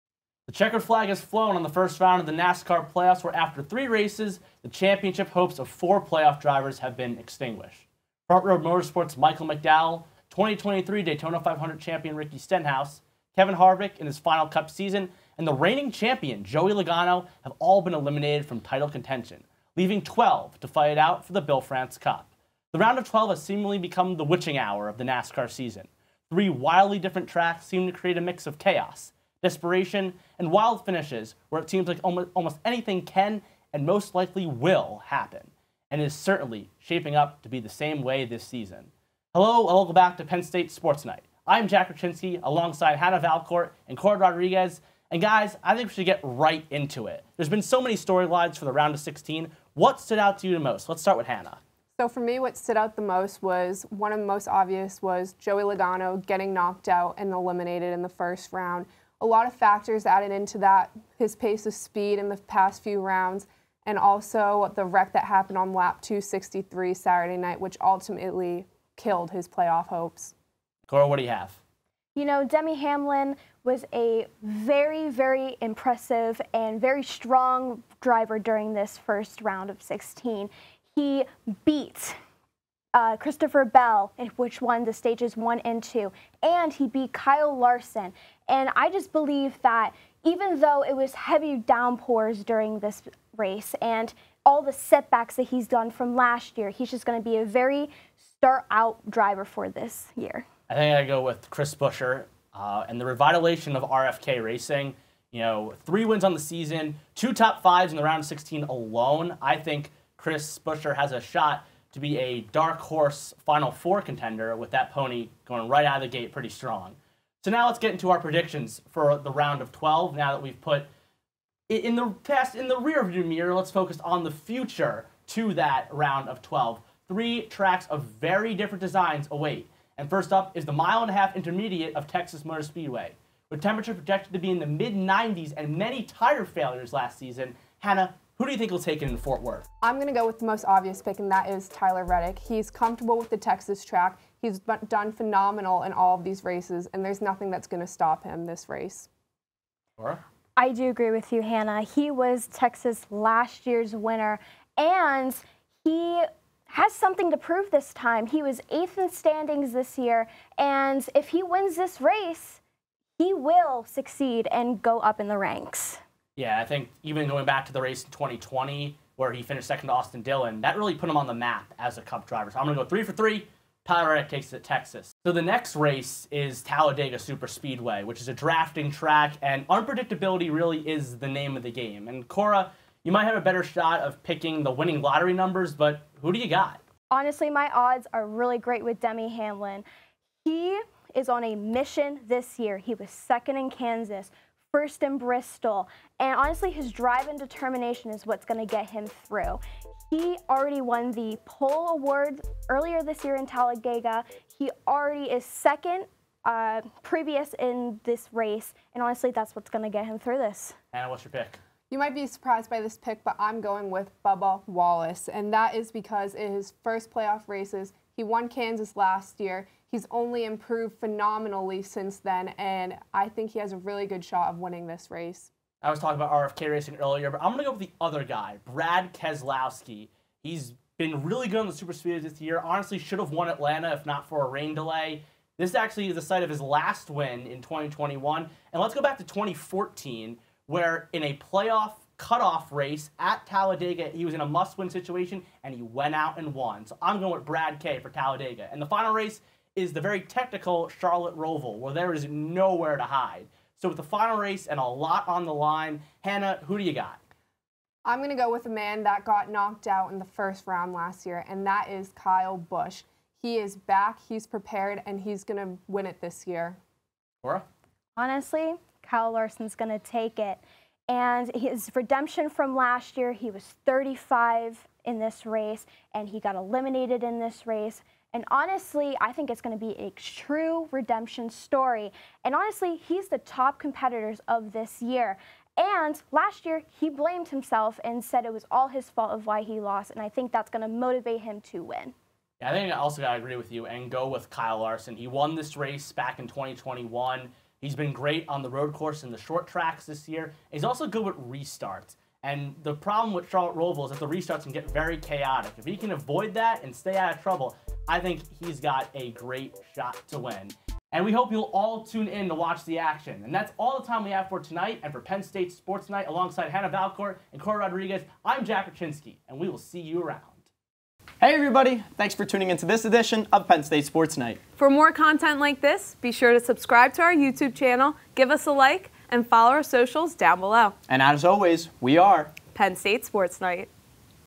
PSSN 2023-24: NASCAR Playoff Predictions
The NASCAR playoffs are underway with the AutoTrader EchoPark Automotive 400 serving as the round of 12 race at Texas Motor Speedway this week. Jack Rachinsky, Hannah Valcourt and Cora Rodriguez discuss what they saw in the round of 16 and what is to come the rest of the way.

Facebook: Penn State Sports Night
TikTok (NEW): Penn State Sports Night

"WE ARE" Penn State Sports Night